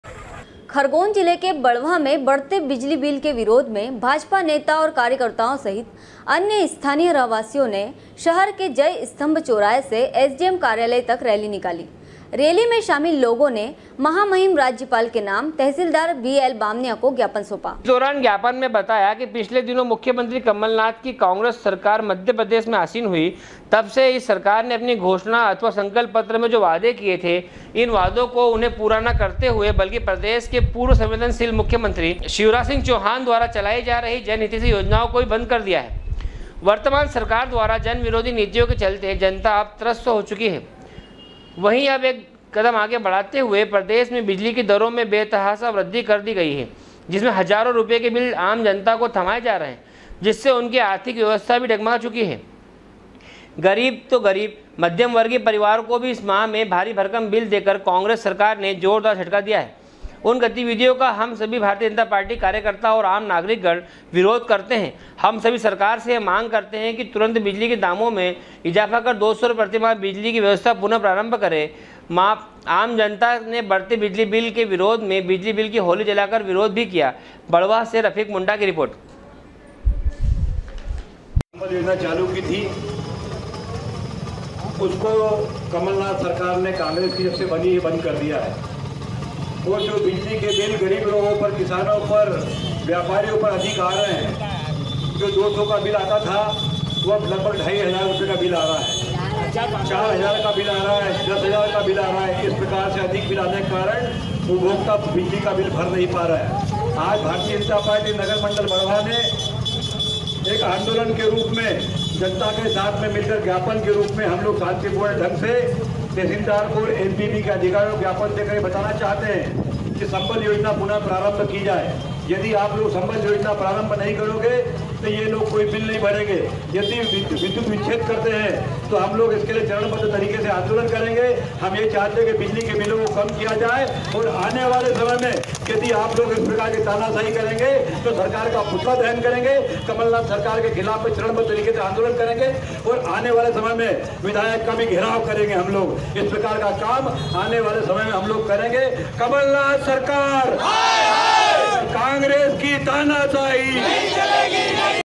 sw24news barwah, 17.07.2019
खरगोन के बड़वाह में बढ़ते बिजली बिल के विरोध में भाजपा नेता और कार्यकर्ताओं सहित अन्य स्थानीय रहवासियो ने शहर के जय स्तम्भ चोराहे से एसडीएम कार्यालय तक रैली निकाली । रैली में शामिल लोगो ने महामहिम राज्यपाल के नाम तहसीलदार बी एल बामनिया को ज्ञापन सौपाsw24news